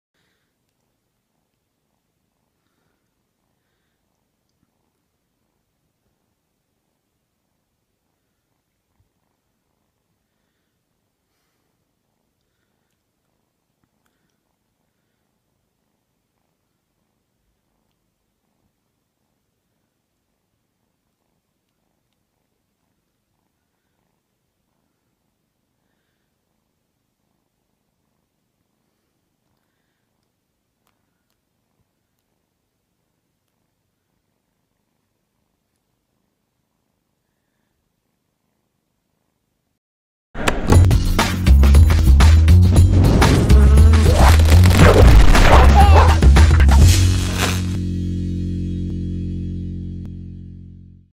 Ruxin plays with ipad
Ruxin the Kitten enjoys playing with his iPad